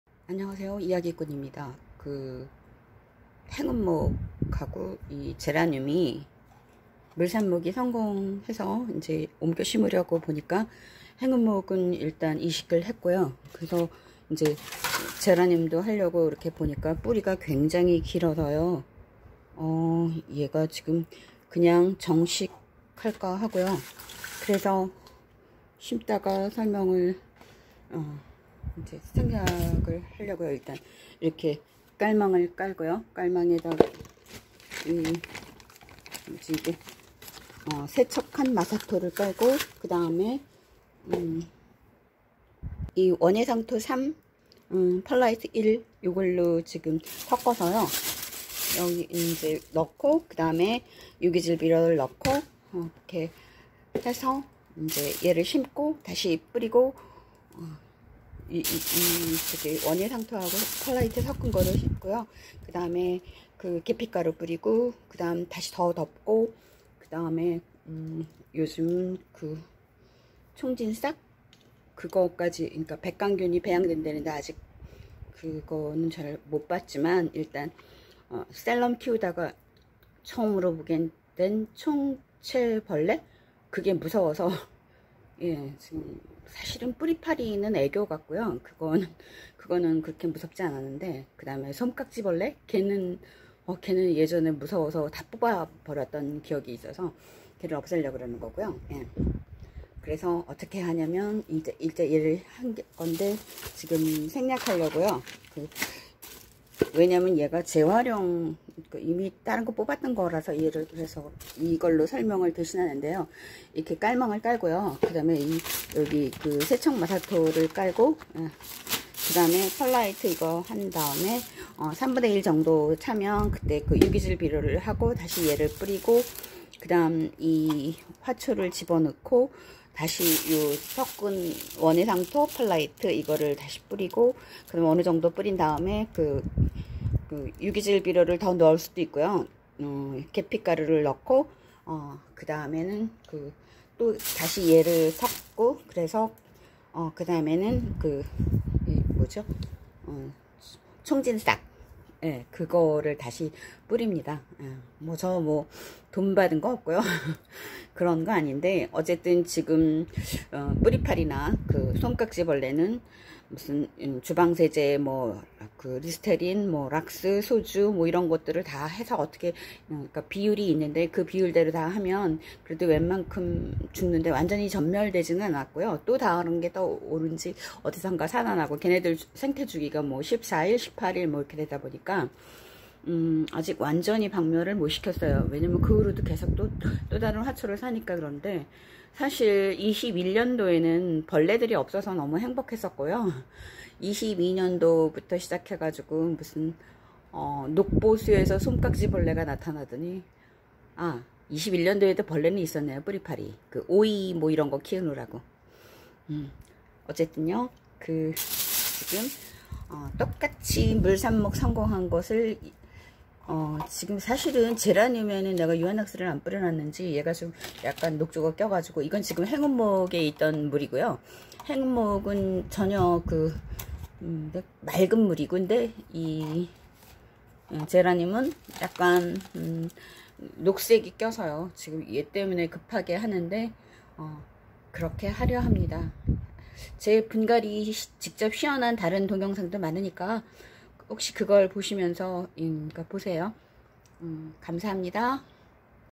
물삽목한 행운목이식하고 제라늄은 정식분갈이
행운목 가지 물삽목한 것 이식하기